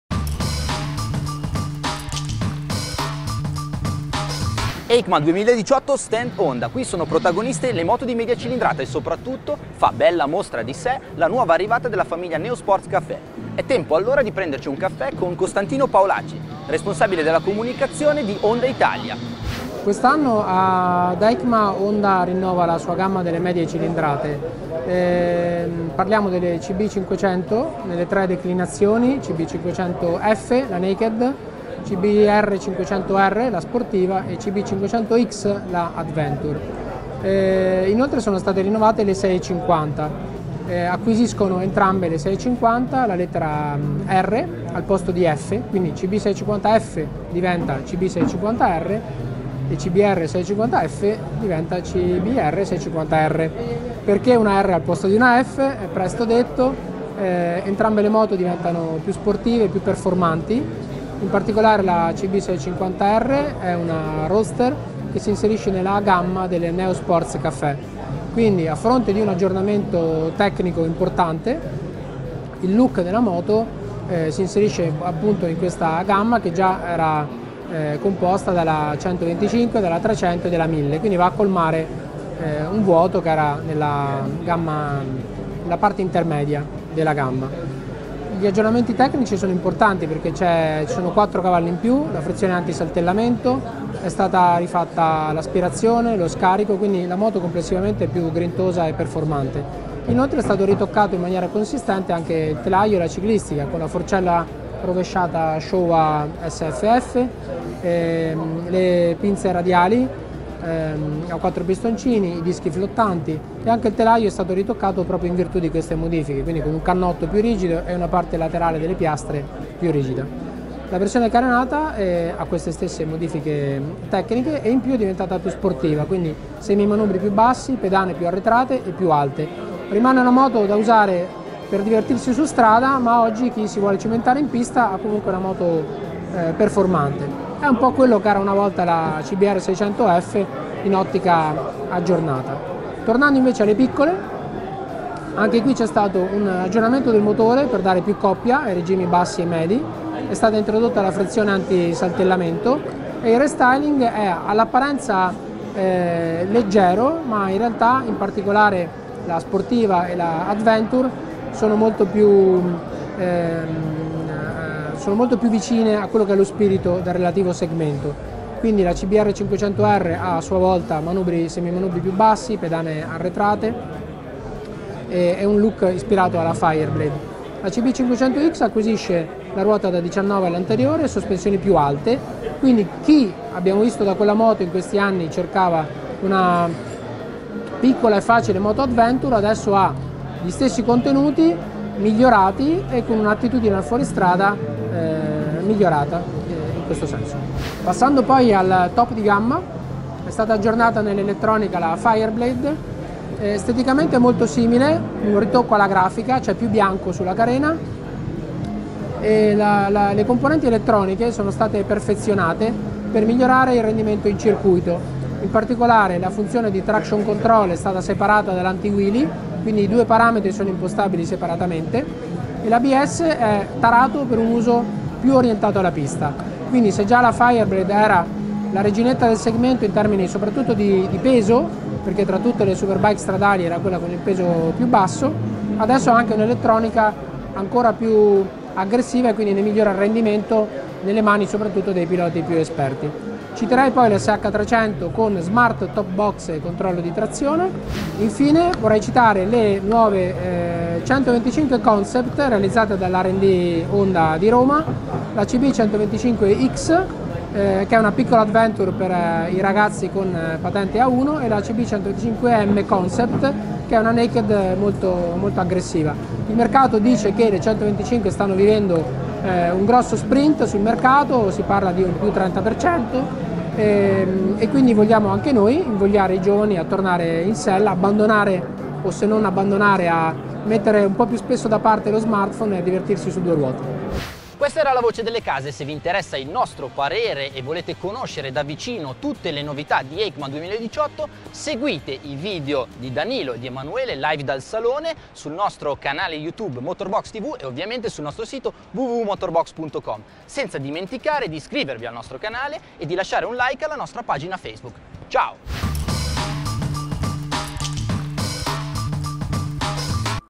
Novità Honda a Eicma 2018 | Le medie cilindrate Honda si rinnovano
La Honda CB650R e le altre novità di Honda a Eicma 2018: ce ne parla Costantino Paolacci, Responsabile Comunicazione Honda Italia.